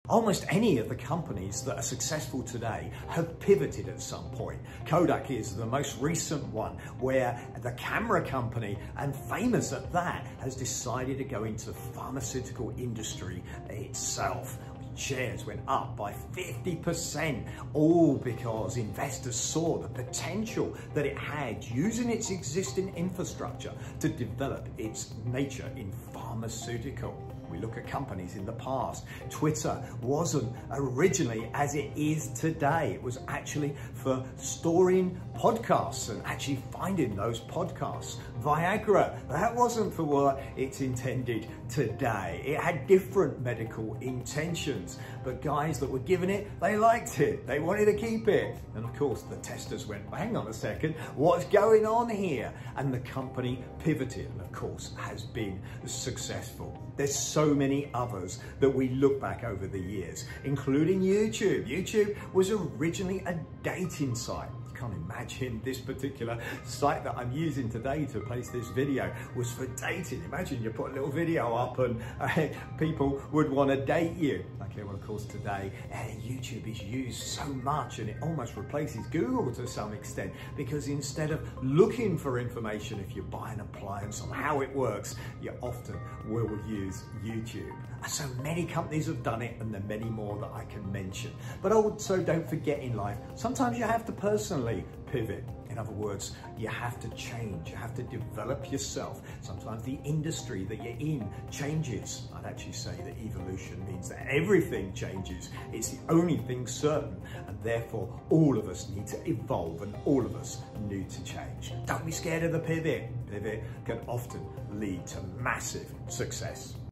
Kodak develop DRUGS! Massive Pivot! Nigel Green deVere CEO
Big news Kodak is to produce drugs . Many companies have pivoted to achieve incredible success . In this short video I explain